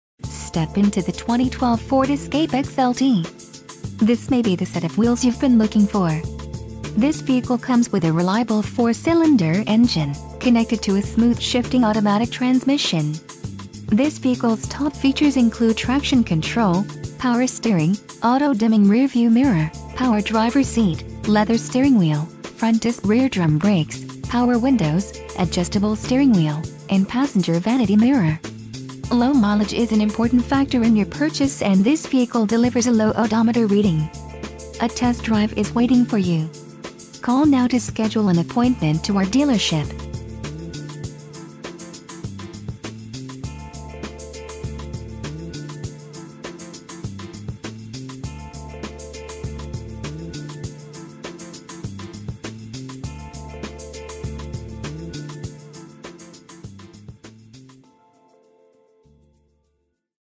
2012 Ford Escape XLT
1 OWNER, AM/FM Single CD/MP3 Capable, CLEAN CAR FAX, Fully automatic headlights, LIKE NEW, LOW MILES, Premium Cloth Buckets w/60/40 Split Rear Seat, and SIRIUS Satellite Radio. How would you like driving away in this terrific-looking 2012 Ford Escape at a price like this? An Intellichoice Best Overall Value 2008. Have one less thing on your mind with this trouble-free Escape.